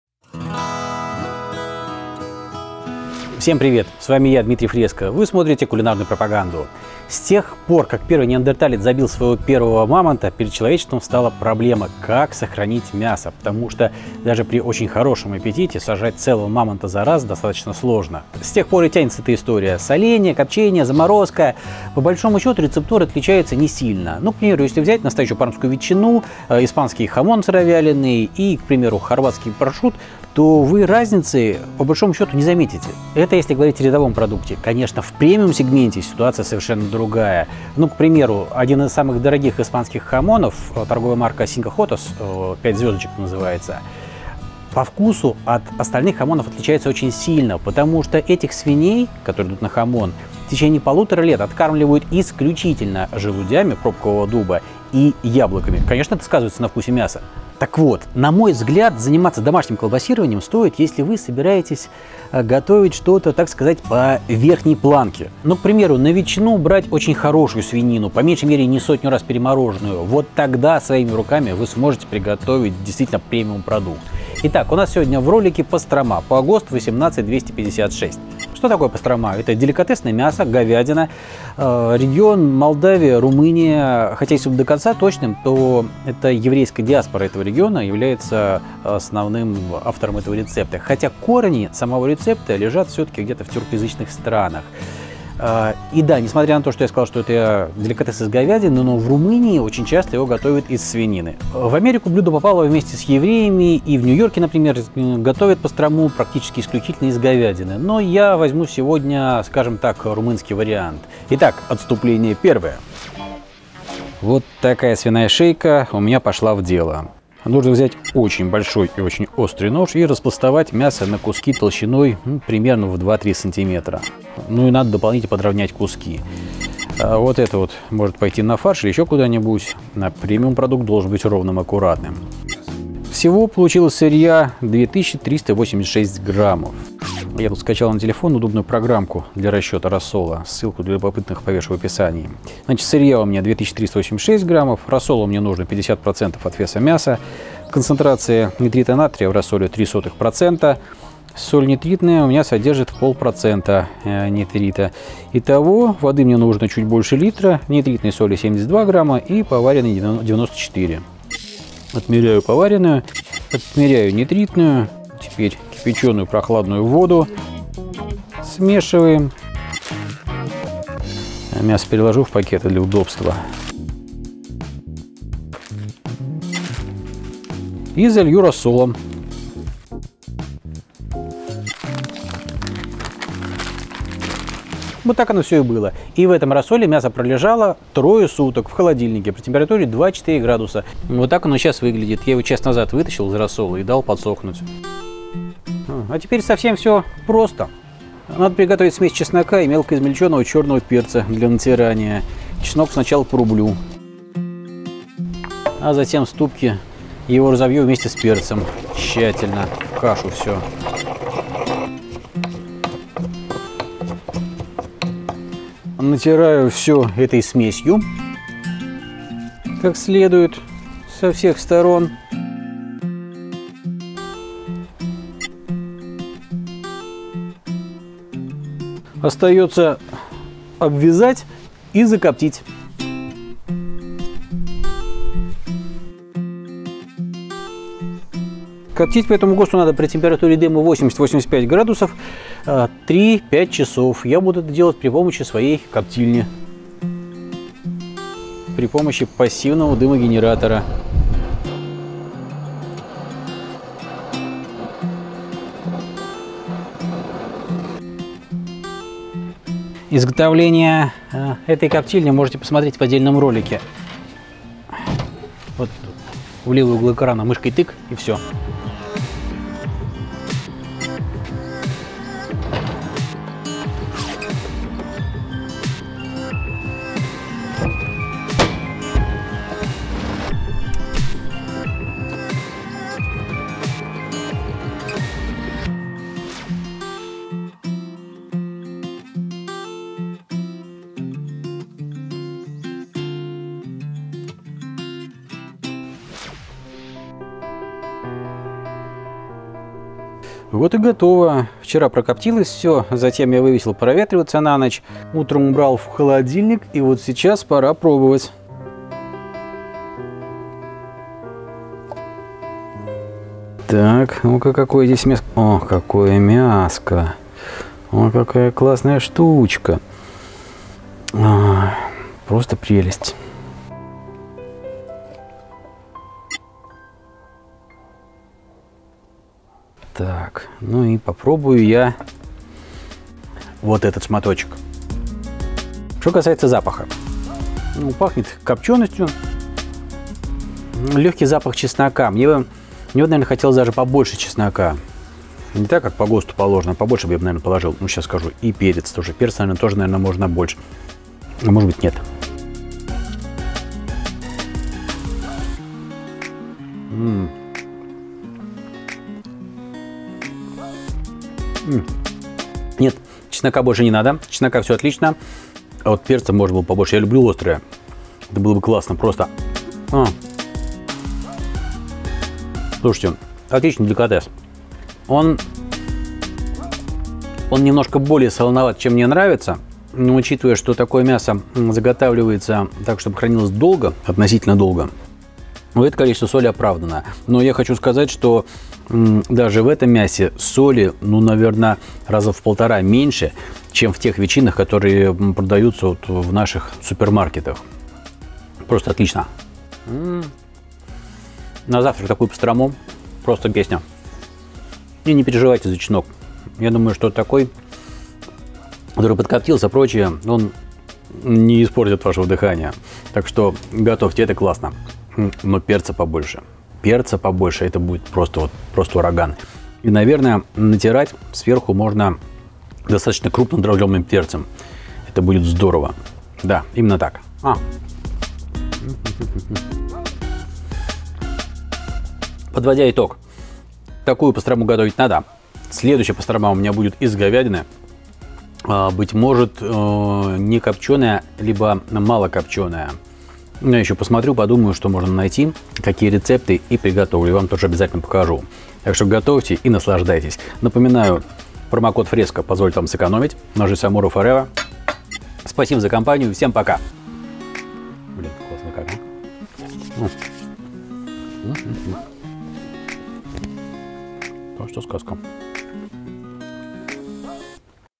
МОЩНАЯ чесночно-перечная ПАСТРОМА по ГОСТ-18256
Свиная шейка 1кг
Рассол плотностью 1,1г/см3 - 500г, (50% от веса мяса) из них
Вода 430мл
Соль поваренная 39г
Соль нитритная с содержанием НН 0,5% - 30г

Чесночно-перечная смесь - 27г, из них
Чеснок свежий 25г (92,5%)
Перец черный 2г (7,5%)

Произведение «МОЩНАЯ чесночно-перечная ПАСТРОМА по ГОСТ-18256» созданное автором по имени Дмитрий Fresco, публикуется на условиях лицензии «Attribution-NonCommercial-NoDerivs» («Атрибуция — Некоммерческое использование — Без производных произведений») 3.0 Непортированная.